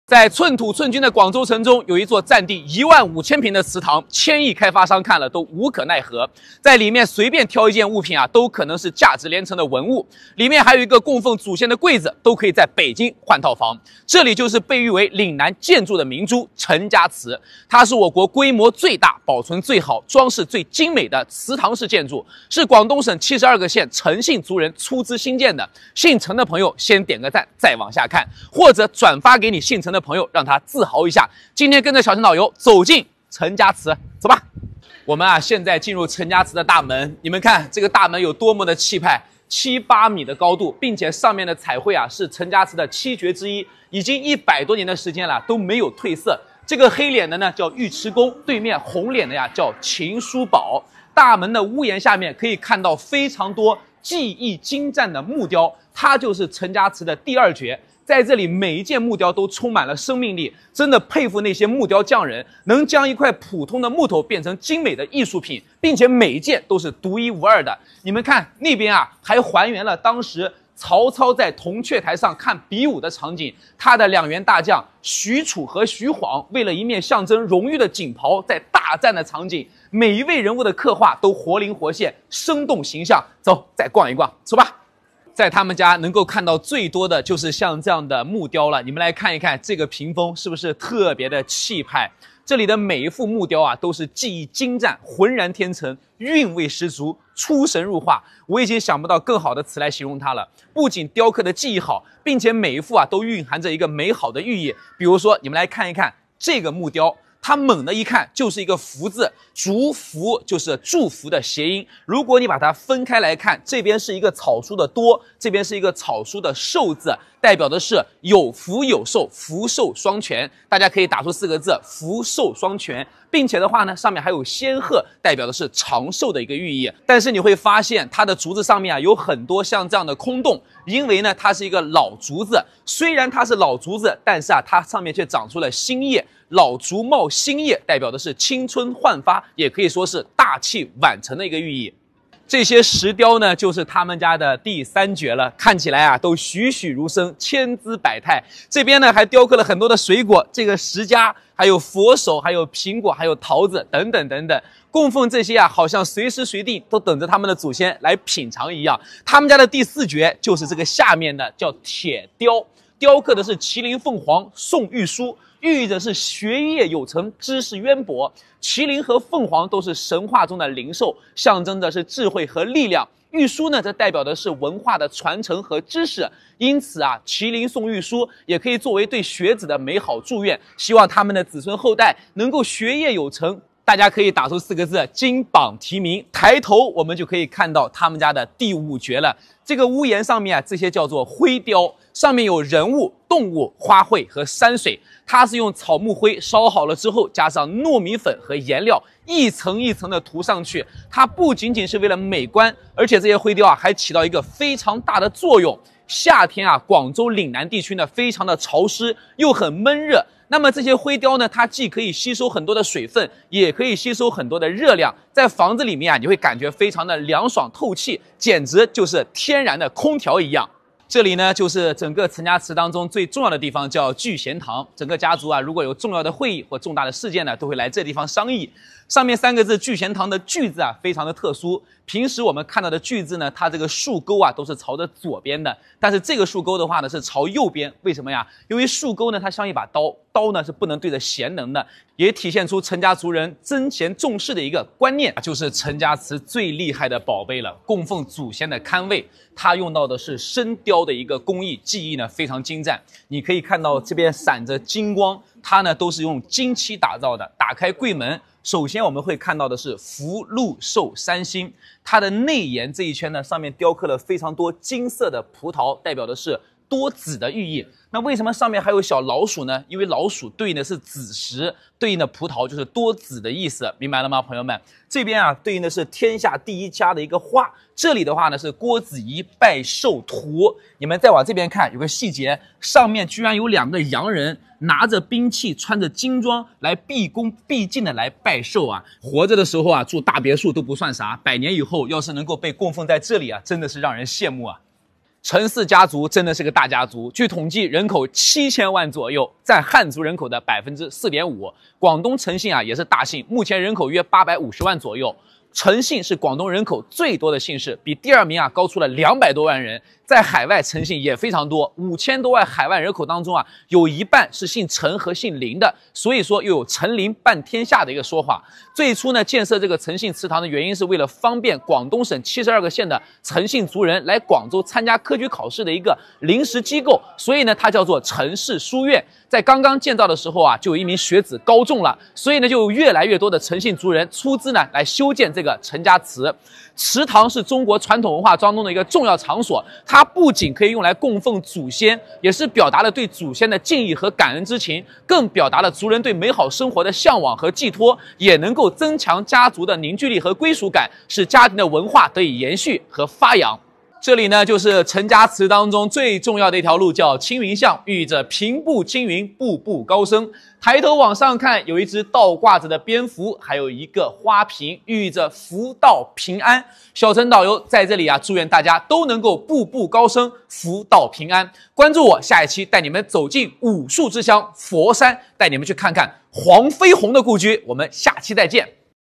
姓陳的也太厲害了，廣州陳家祠簡直就是祠堂中的天花板
大家好，我是小程導遊！ 一個全網1600萬粉絲的「網紅導遊」用故事傳遞文化，用文化傳遞正能量！